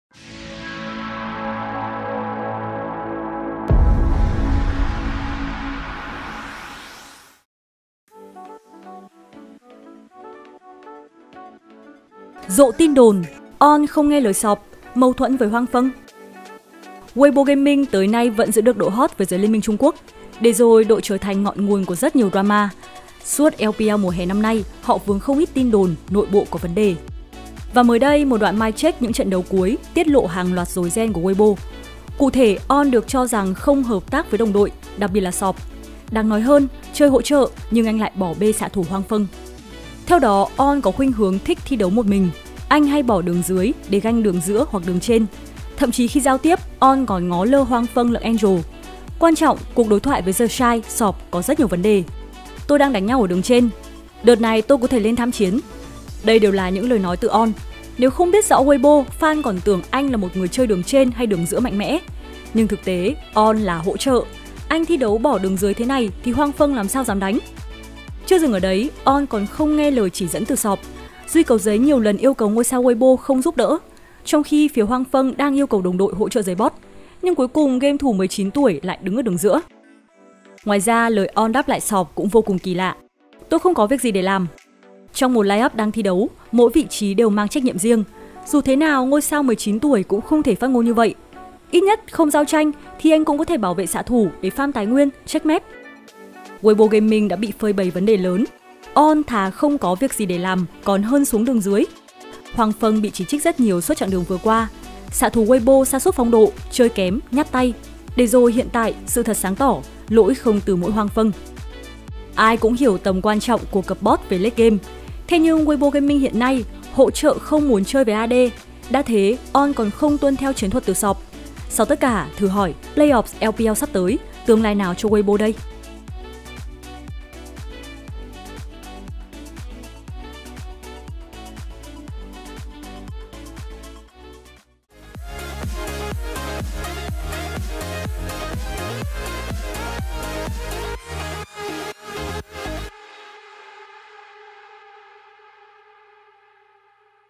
LMHT Chém Gió: Tin đồn ON không nghe lời SofM, mâu thuẫn với huanfeng?
WBG tới nay vẫn giữ được độ hot với giới LMHT Trung Quốc. Để rồi, đội trở thành ngọn nguồn của rất nhiều drama. Suốt LPL mùa Hè 2022, họ vướng không ít tin đồn nội bộ có vấn đề.

Và mới đây, một đoạn mic check những trận đấu cuối tiết lộ hàng loạt rối ren của WBG. Cụ thể, ON được cho rằng không hợp tác với đồng đội, đặc biệt là SofM. Đáng nói hơi, chơi hỗ trợ nhưng anh lại bỏ bê xạ thủ huanfeng.